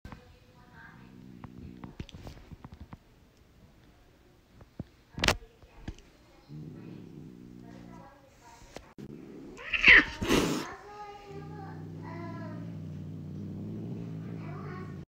Mom Siamese cat mad at rescue tortie kitten
This baby kitten appeared near our trash can one day and was very feral but after a while she calmed down,my cats however did not like her from the start.after about two weeks she disappeared and I’m guessing went back with her owners
This video shows my female cat attack baby rescue kitten(about 2-3 months old)

Kitten suffered no injuries.
Mom was just jealous but i am in no way trying to get her upset.they also did not see each other anymore once i noticed they did not get along.